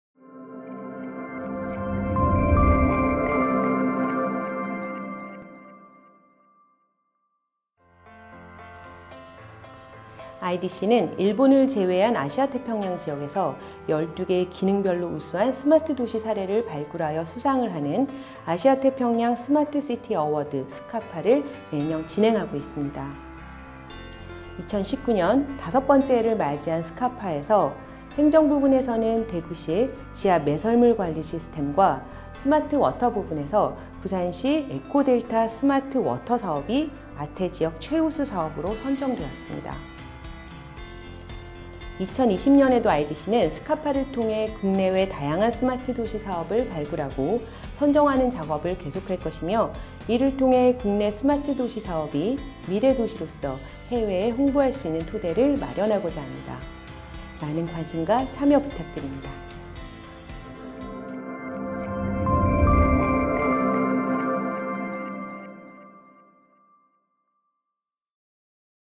국내외 우수 스마트 시티 선정을 위한 2020년 아시아 태평양 스마트 시티 어워드(SCAPA) 후보 추천 진행 중! (~2월 14일까지)
IDC는 일본을 제외한 아태지역에서 우수 스마트 시티 사례를 발굴하여 수상하는 아시아 태평양 스마트 시티 어워드(Smart City Asia/Pacifc Awards, SCAPA)를 매년 진행하고 있습니다. 올해로 여섯 번째를 맞이한 SCAPA 2020에서는 기능별로 구분한 14개의 스마트 시티 eService 영역에서 뛰어난 스마트 시티 사례를 선정할 예정입니다. 미래 도시로서 해외에 홍보할 수 있는 좋은 기회이오니 다양한 국내 스마트 시티 프로젝트를 추천 부탁드립니다.